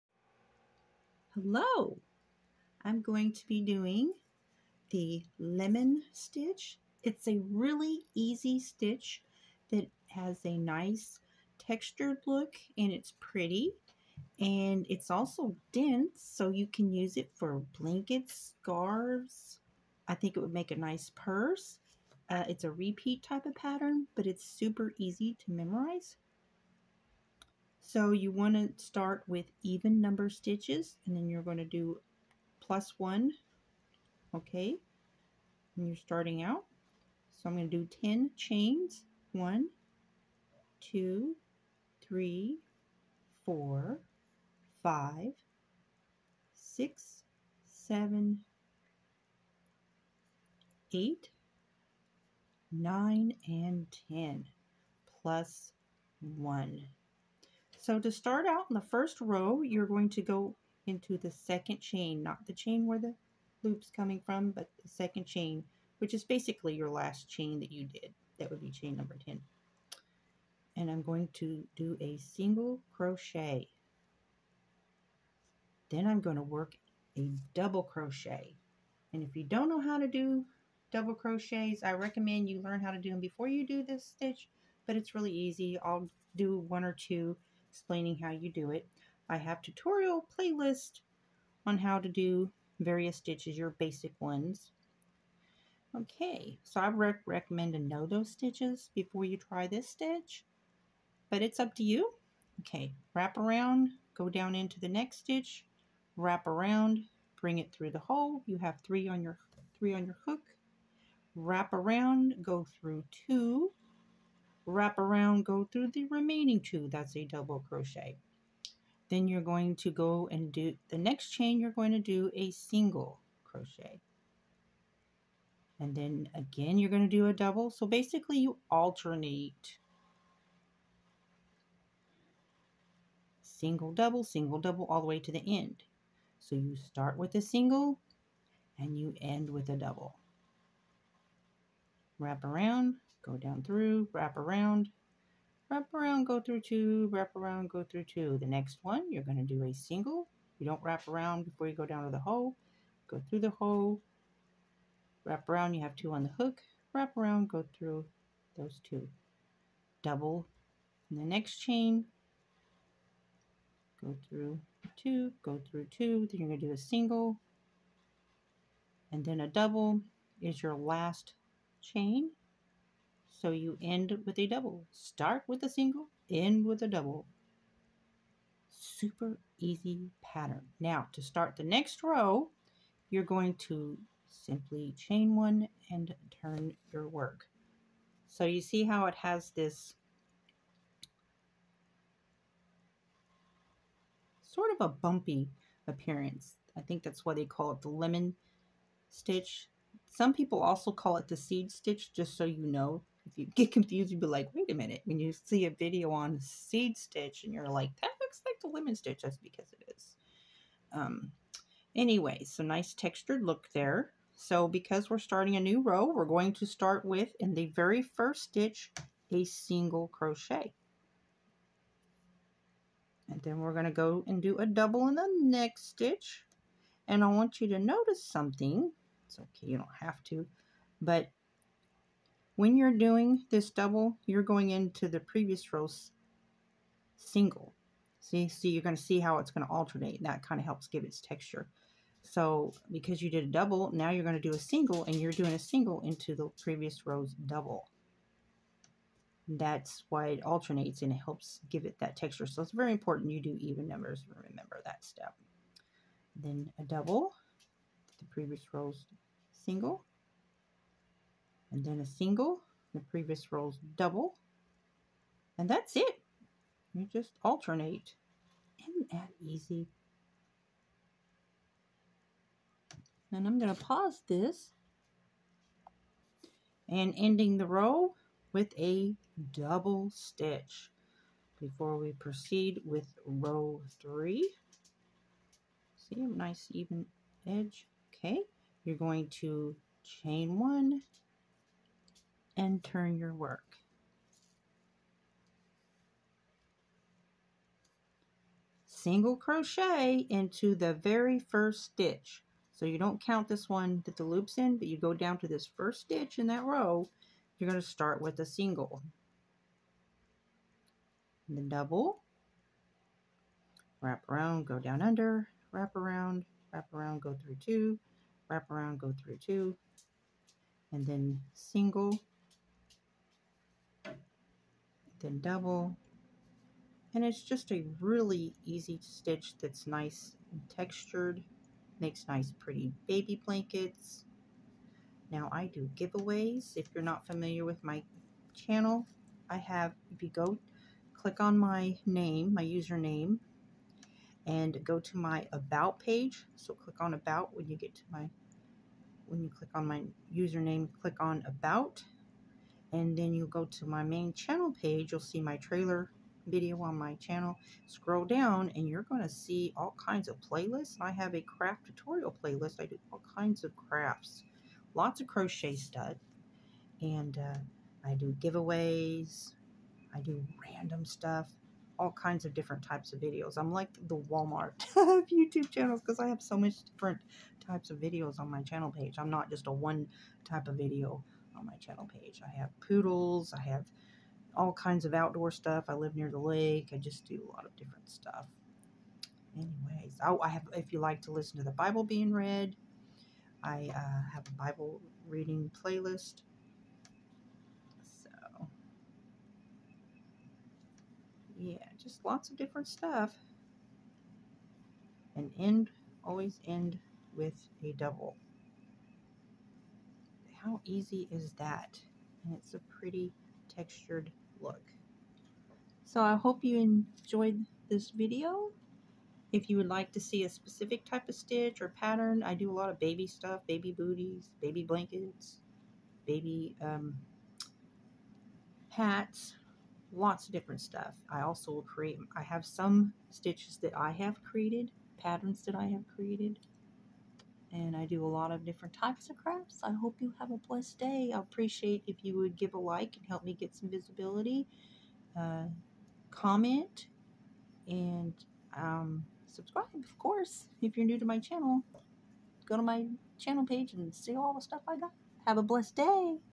CROCHET LEMON STITCH TUTORIAL EASY AND PRETTY!
Sharing how to do the Lemon stitch also known as the seed stitch. Very easy stitch to do with nice texture, and density for various projects!